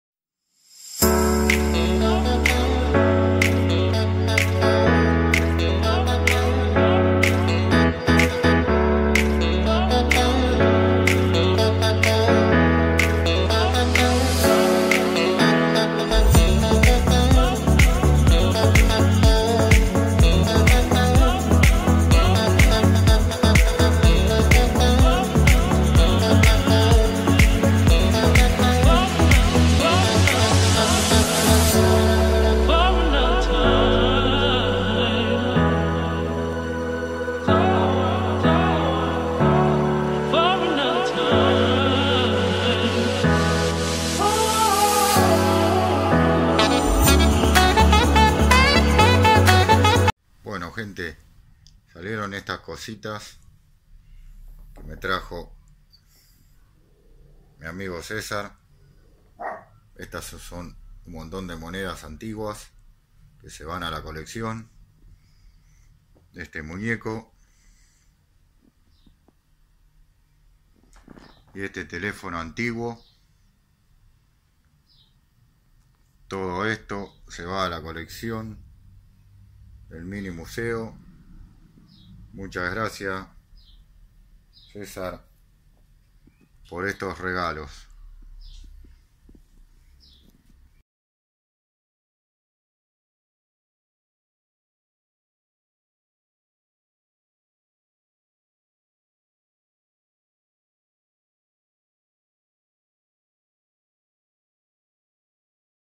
y seguimos sumando cositas 👏👏
muchas gracias cesar por los regalos seguimos sumando el mini museo cada ves  se pone mejor .
mil gracias a todos los que siempre me dan cosas para el mini museo y agredesco a todos los suscriptores por el aguante